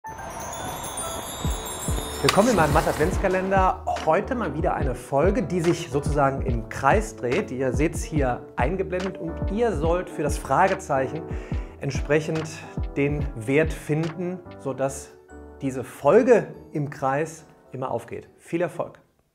🎄✨🎁 MATHE by Daniel Jung-ADVENTSKALENDER TAG 22: 🎁✨🎄
🎄✨🎁 MATHE-ADVENTSKALENDER TAG 22: 🎁✨🎄

Welche Zahl passt an die Stelle des Fragezeichens, sodass die Folge sich immer im Kreis dreht? (Grafik in Video)

Wer die richtige Lösung in die Kommentare bei meinem IG-Post (@danieljungeducation) schreibt, kann eines meiner berühmten Studyhelp Hefte gewinnen mit dem jede Klausur ein Klacks wird 🤝

Den Gewinner informieren wir per Direct Massage.

Quelle: Nobuyuki Yoshigahara, Puzzles 101